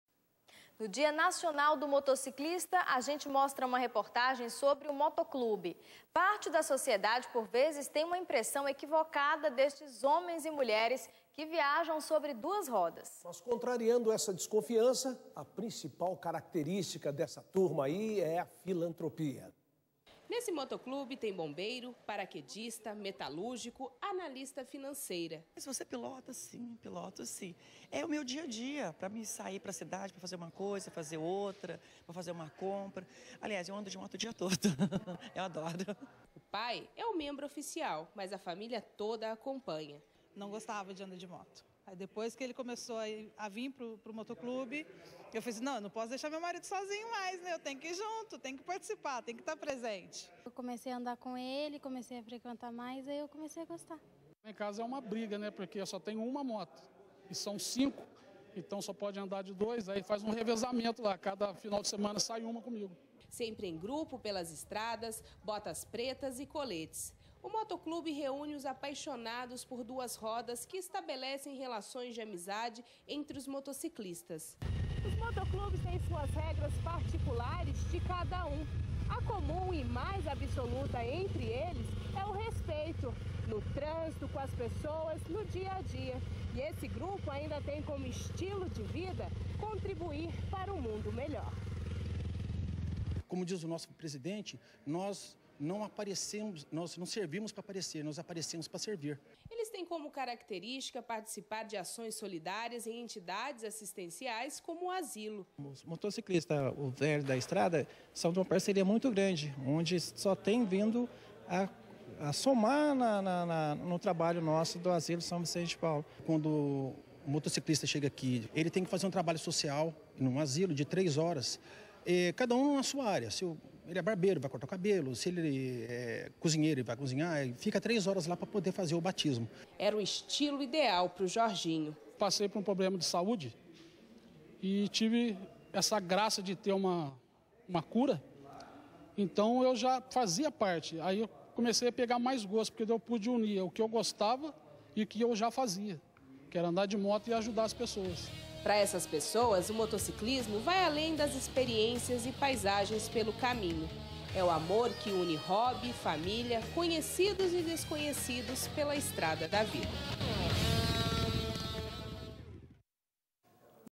Conheça o trabalho realizado por um motoclube no interior de SP
Nesta sexta feira comemora se o dia Nacional do Motociclista, e agora vamos apresentar uma reportagem sobre um motoclube. Parte da sociedade, por vezes, tem uma impressão equivocada desses homens e mulheres que viajam sobre duas rodas. Mas contrariando essa desconfiança, a principal característica dessa turma aí é a filantropia.

Reportagem de Elaine Santos e Tony Cursino.